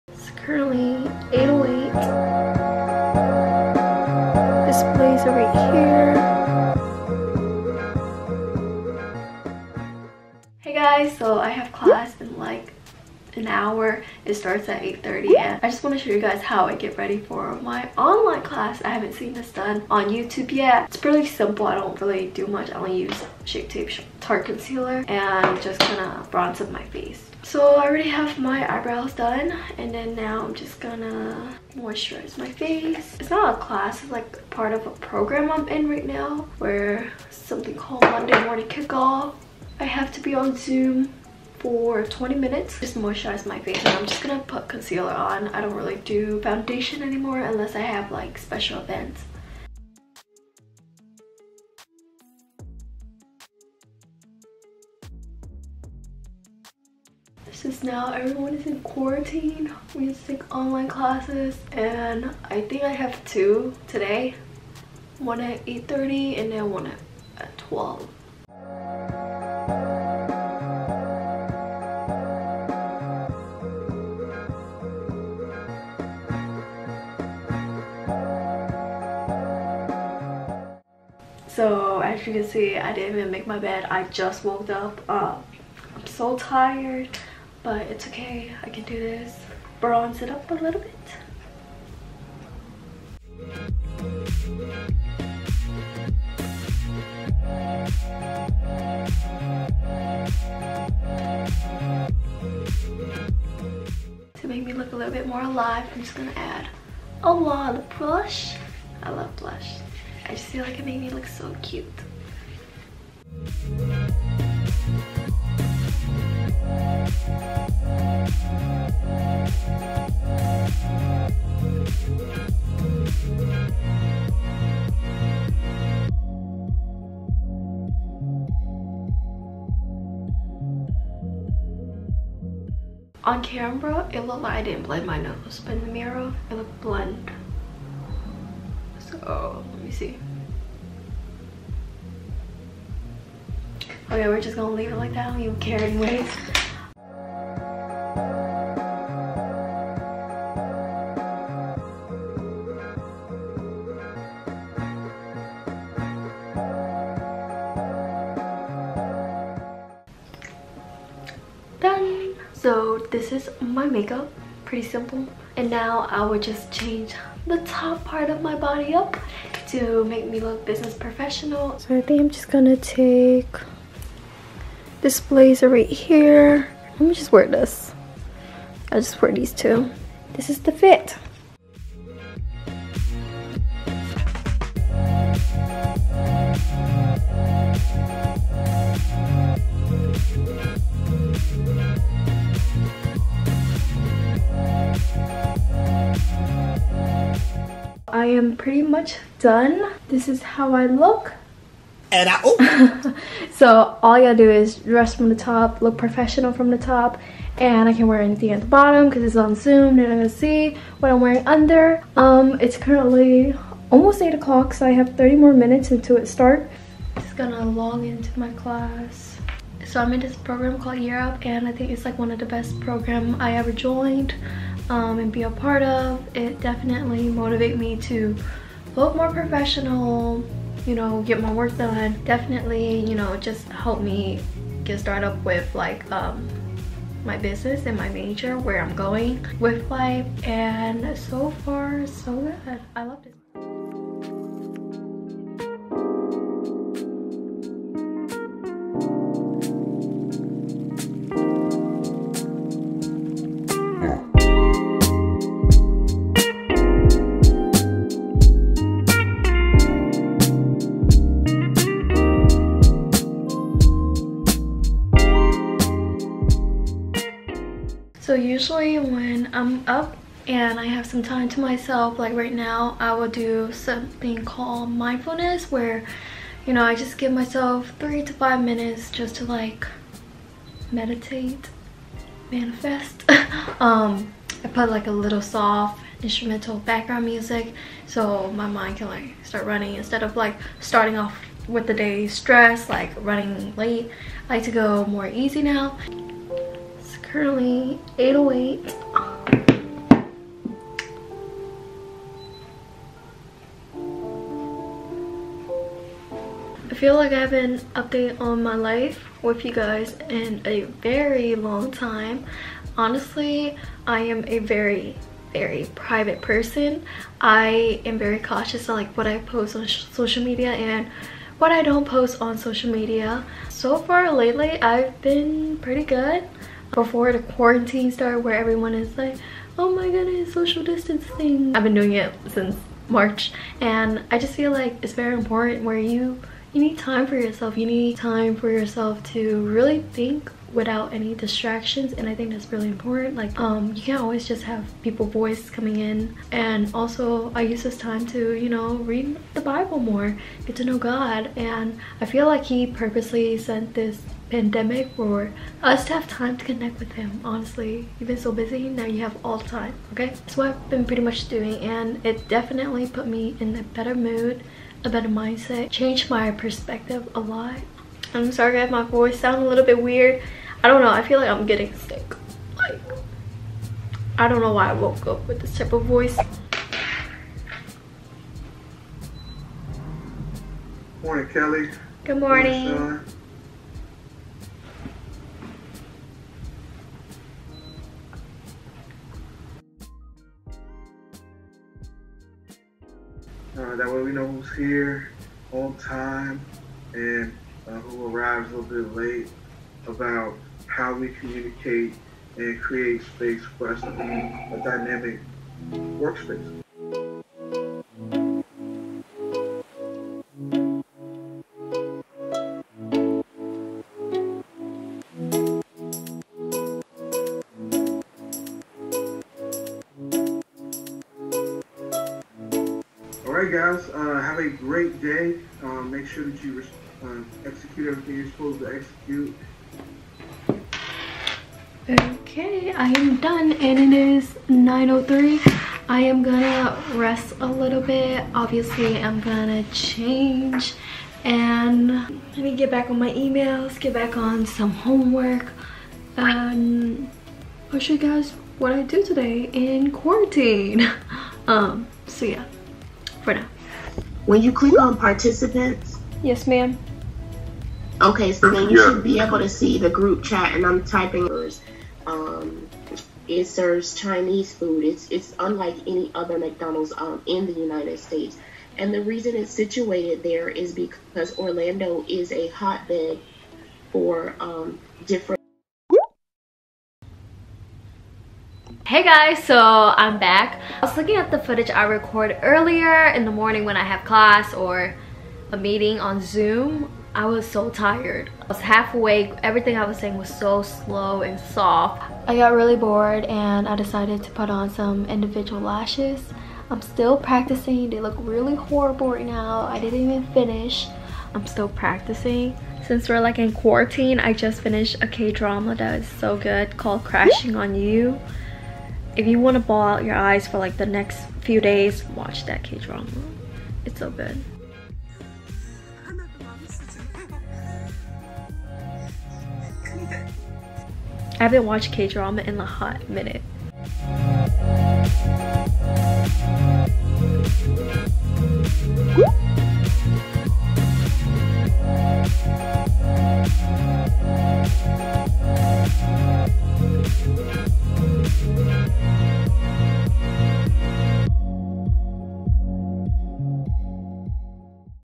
8AM College Class QUARANTINE Edition
Get ready with me for online classes and school lol
be safe. practice social distancing